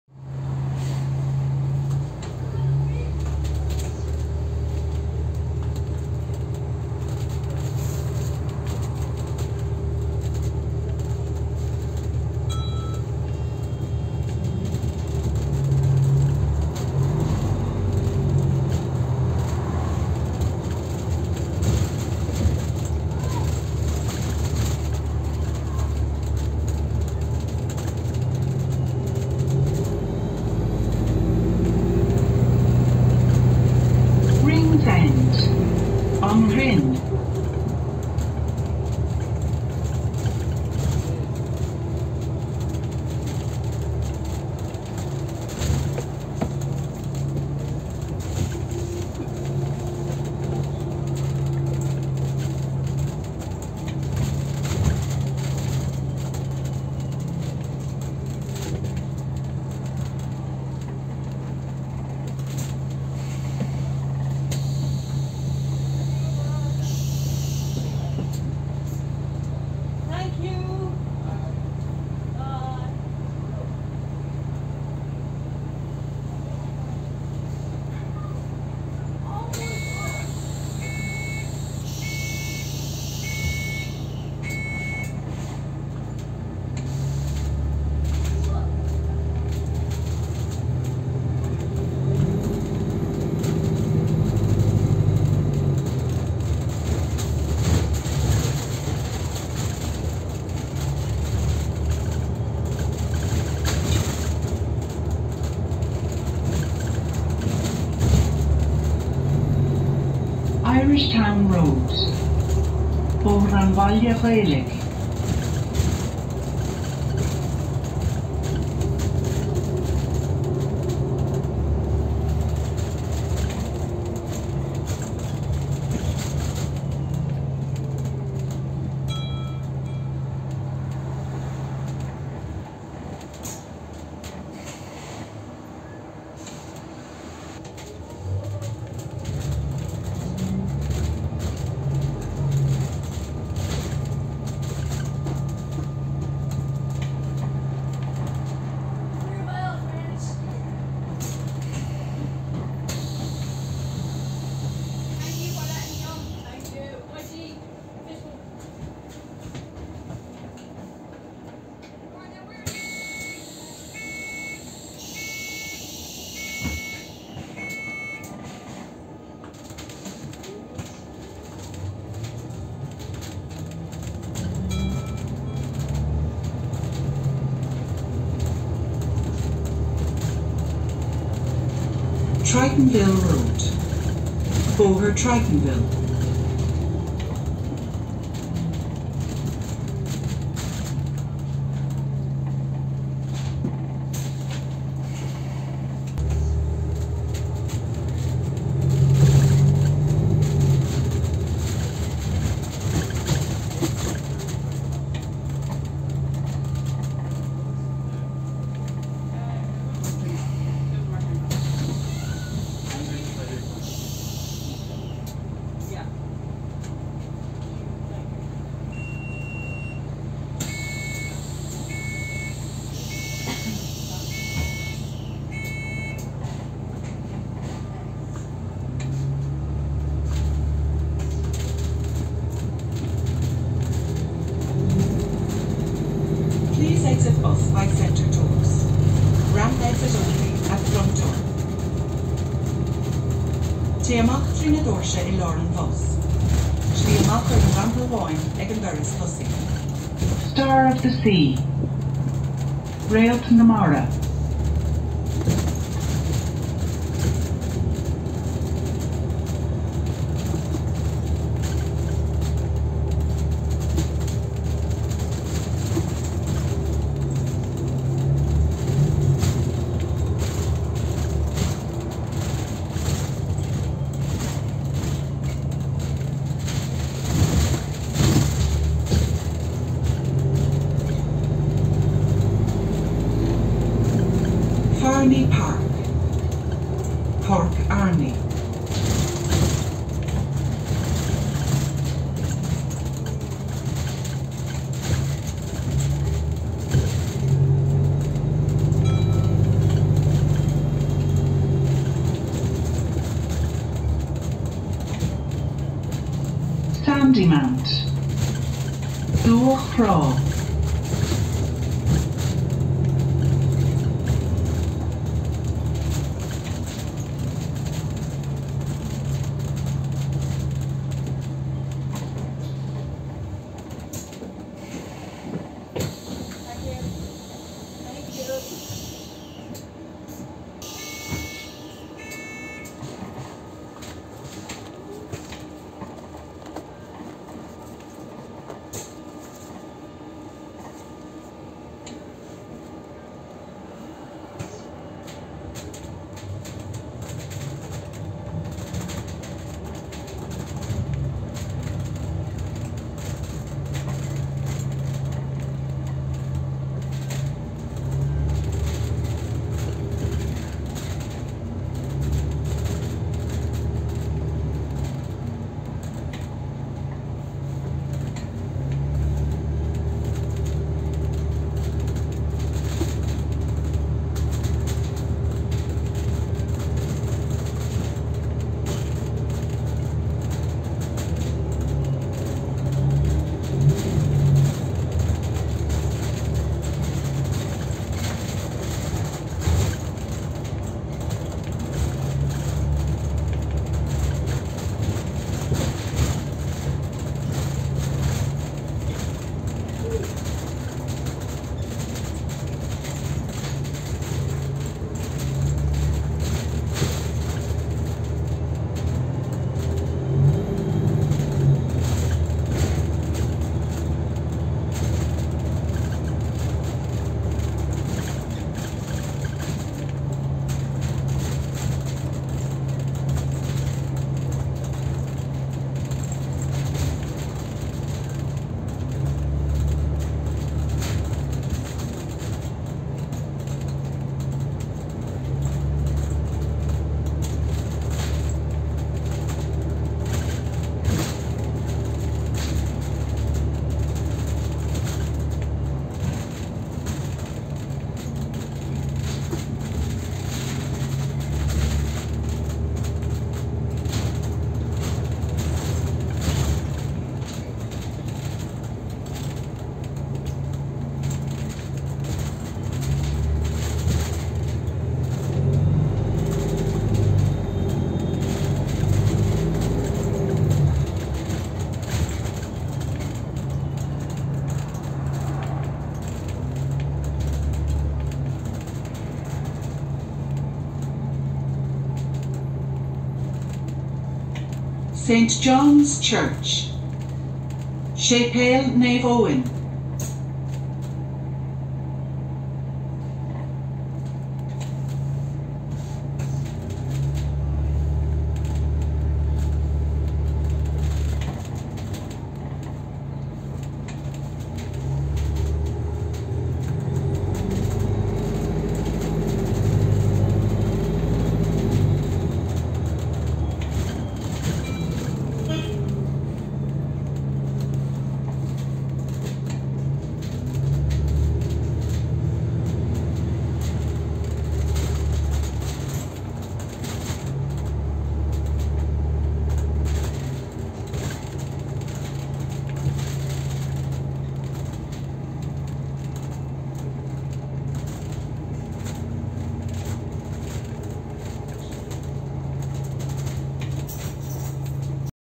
*C-Spine* Dublin Bus | Enviro400ER Cummins Hybrid | PA143 (212-D-17501) | C1 to Sandymount
Welcome to a new Premiere! Today, we have a Dublin Bus Hybrid (PA143) on the C1 service to Sandymount, we are riding it from Ringsend Garage to the last stop in St. John's in Sandymount. I rode a lot of the PA140's that day for some reason XD. This bus is cool, also to note we saw a PA on the 47 passing by. There's not much other to say about the bus. The route C1 replaced route 25A and a part of the route 1 on the 28th of November 2021.

Operator: Dublin Bus
Bus: Alexander-Dennis / Enviro400ER
Registration: PA143 (212-D-17501)
Route: C1 to Sandymount
Livery: 2021 Green/Yellow TFI Livery
Garage: Phibsboro
Date: January 29, 2022